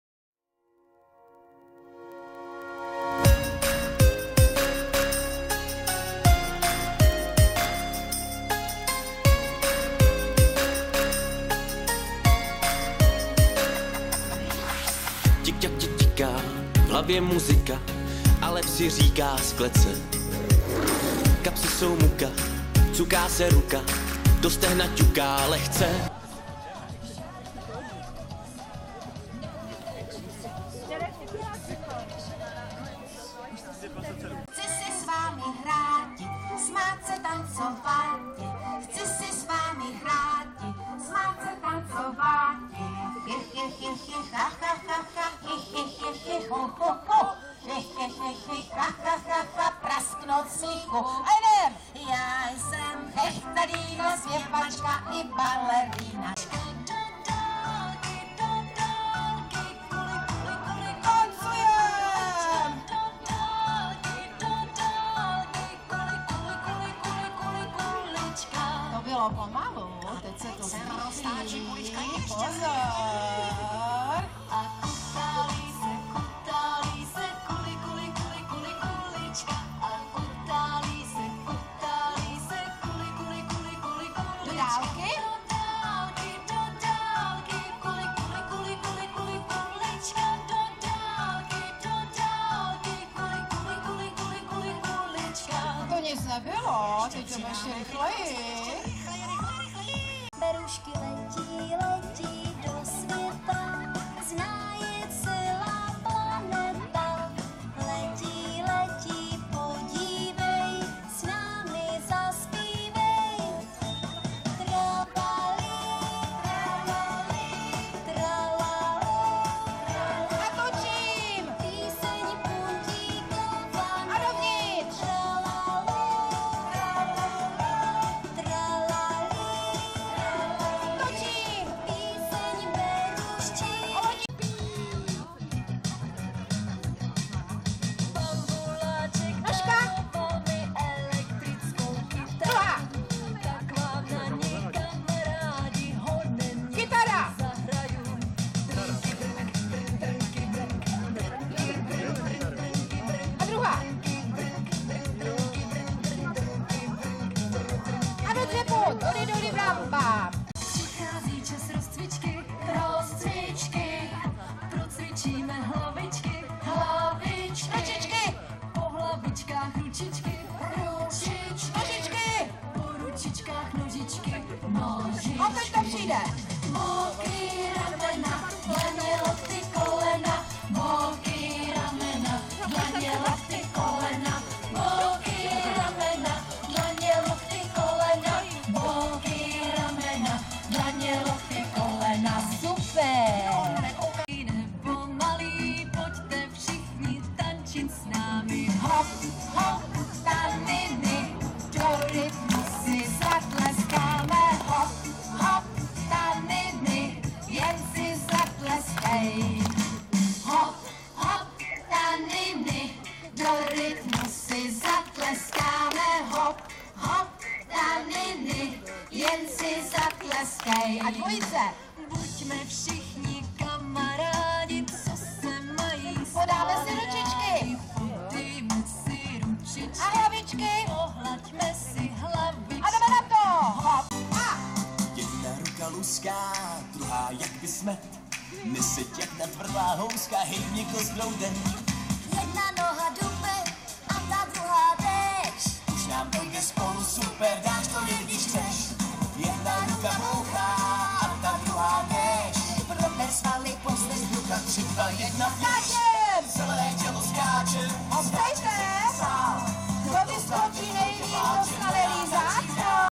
Pohádková cesta za zdravím v Hrabové 5. 9. 2020.
Již třetí ročník Pohádkové cesty v Hrabové. Závěrečné tanečky zakončily úspěšnou a hojně navštívenou kulturní akci, která přitáhla a spojila všechny účastníky bez rozdílu věku a názorů.